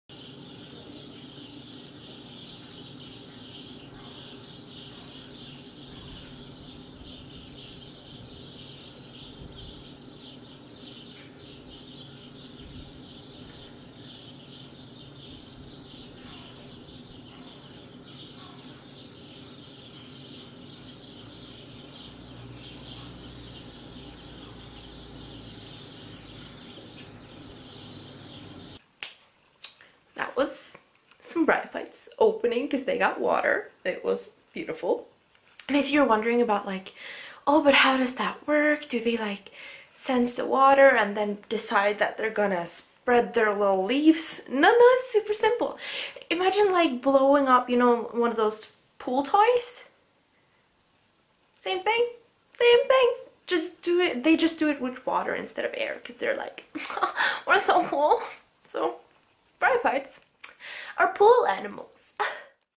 Unfolding moss- from Grey to Green
Isn't it beautiful! This is a fast forward of the ten minutes it took the moss to fold out its wings! Okay leaves, but I'm still gonna imagine them as tiny angels.